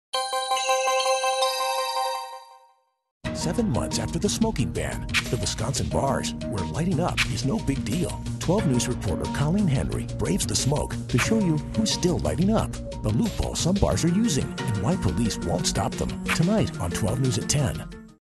Tonight At 10: Bars Still Letting Customers Light Up
Smoking is banned in bars around Wisconsin, but that doesn't mean it's gone.